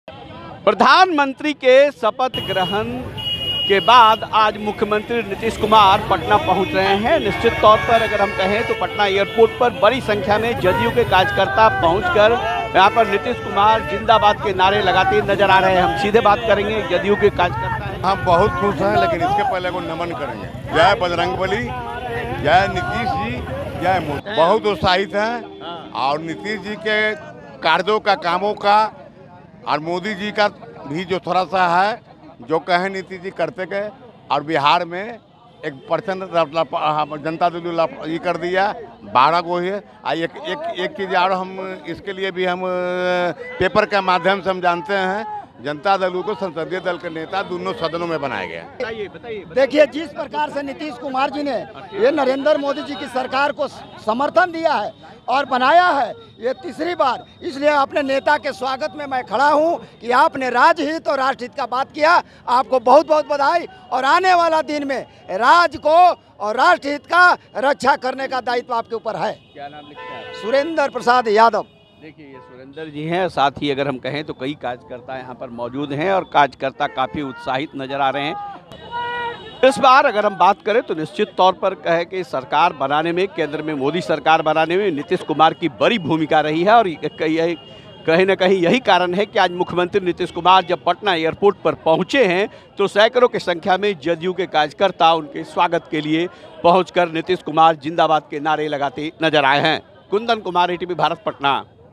PM Modi के शपथ ग्रहण के बाद पटना लौटे CM Nitish Kumar, स्वागत में जुटी भीड़ | Bihar News
PM Modi Oath Ceremony: प्रधानमंत्री नरेंद्र मोदी के शपथ ग्रहण के बाद बिहार के मुख्यमंत्री नीतीश कुमार आज पटना लौट आए हैं. CM Nitsh Kumar पिछले पांच दिनों से दिल्ली में थे. Modi Cabinet 3.0 में जेडीयू कोटे से ललन सिंह और रामनाथ ठाकुर को भी शामिल किया गया है.

About ETV Bharat Bihar:-

You Can follow ETV Bharat Bihar ( ईटीवी भारत बिहार ) On :-